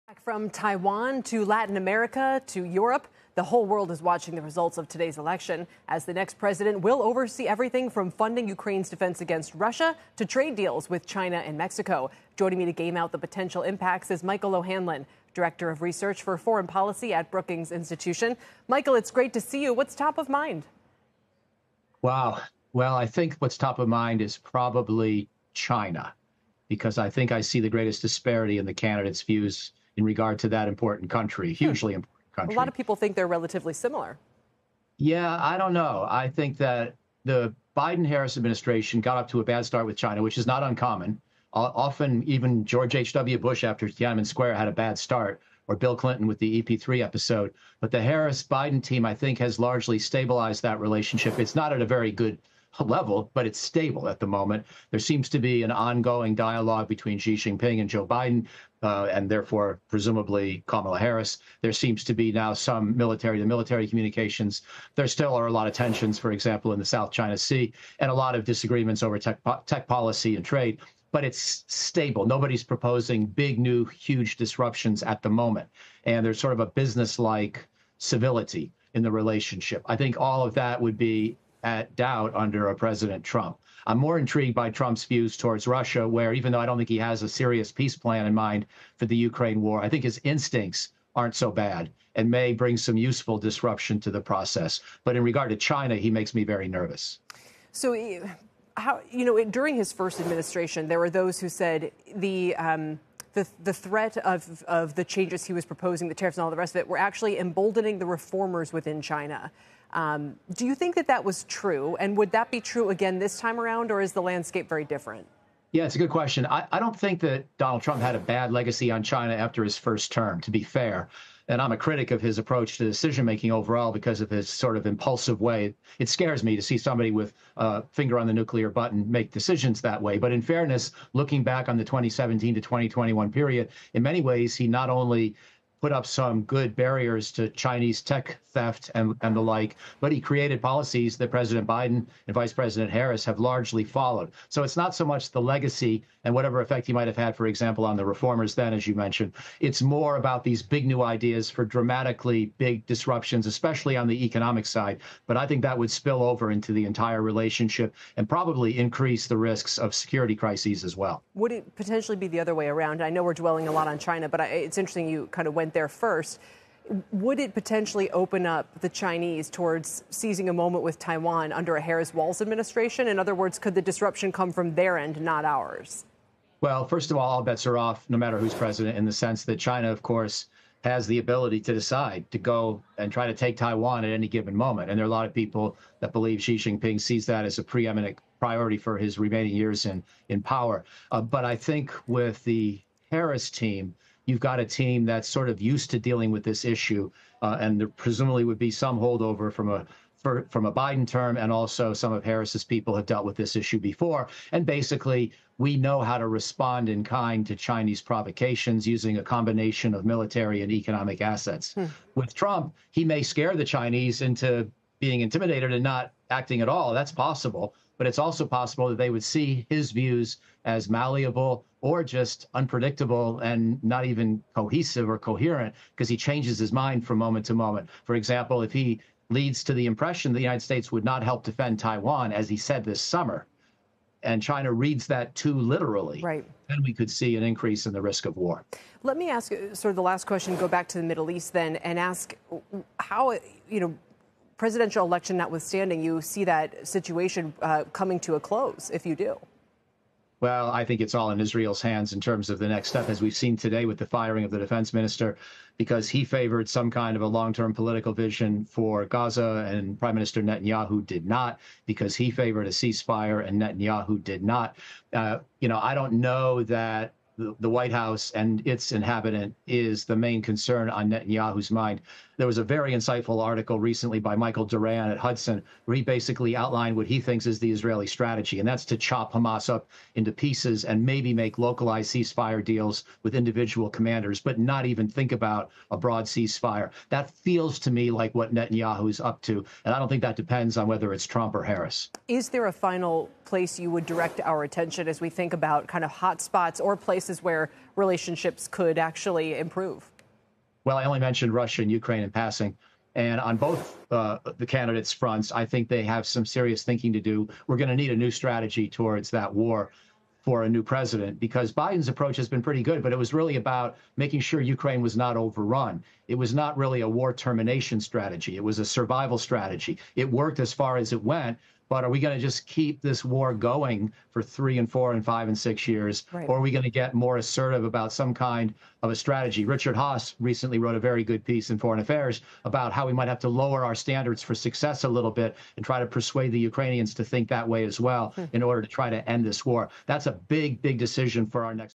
China relations 'top of mind' in election, says Brookings' Michael O'Hanlon
Michael O’Hanlon, Brookings Institutions director of foreign policy research, to discuss what foreign relations are most at stake for the U.S. election, how China relations could play out in either election outcome, and more.